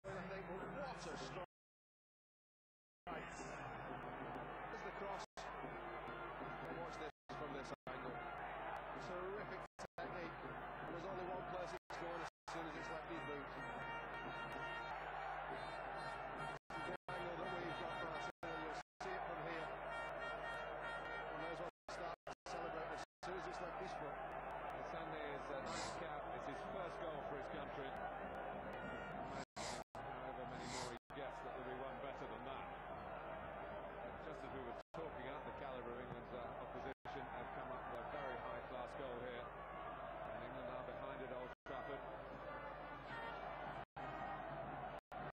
02.07.2007 Inglaterra - España, gol de Iniesta
El único gol del partido amistoso de 7-2-2007 entre los equipos nacionales de Inglaterra y España. España ganó con 0-1.